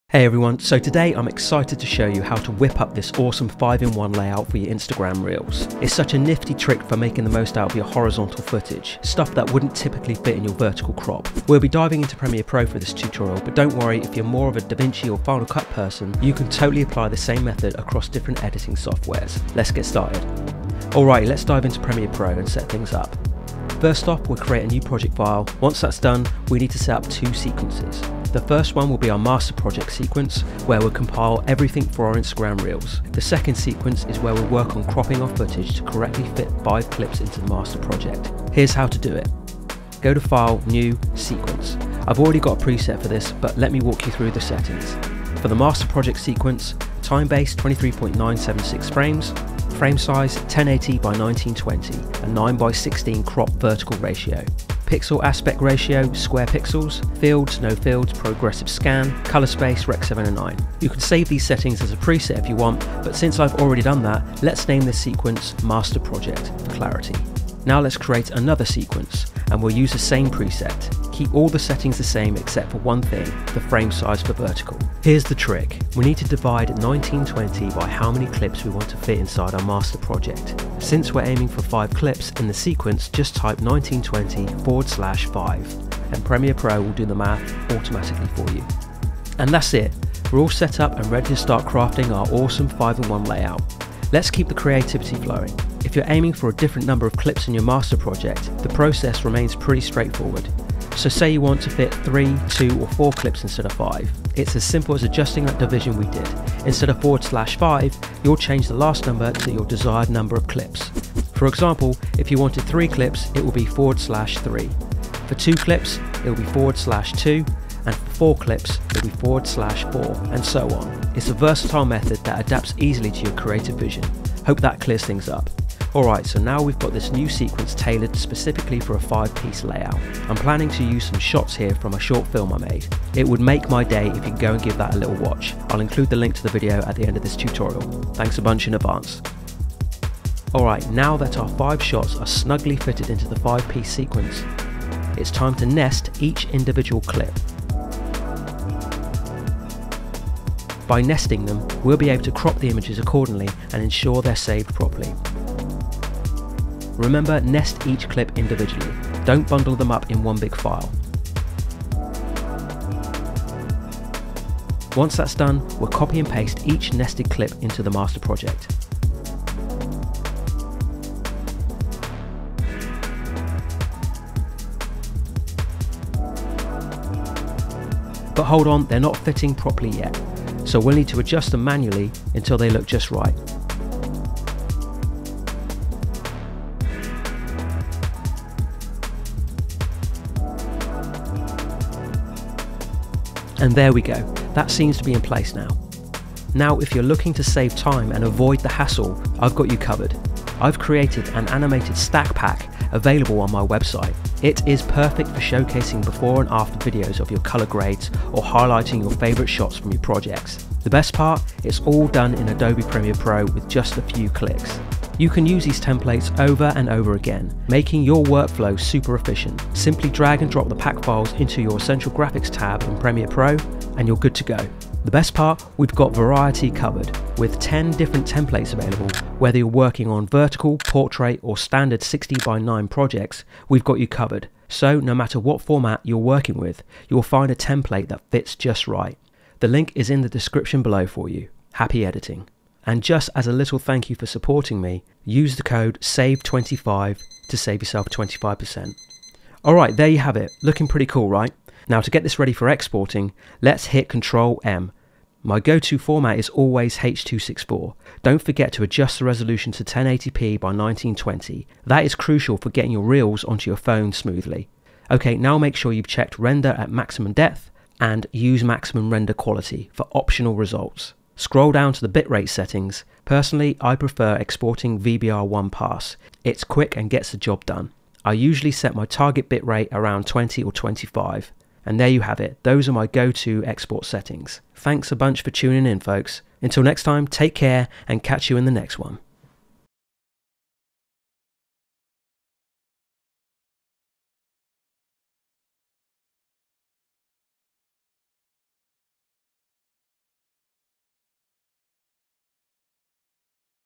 5 in 1 Reels Tutorial
Main Camera
My main Gimbal
My action Cam

0:00 Intro
0:22 set up sequence in premiere pro
1:40 How to divide a short to fit 5 stacked videos
3:16 Splitscreen pack
4:10 Export settings for instagram reels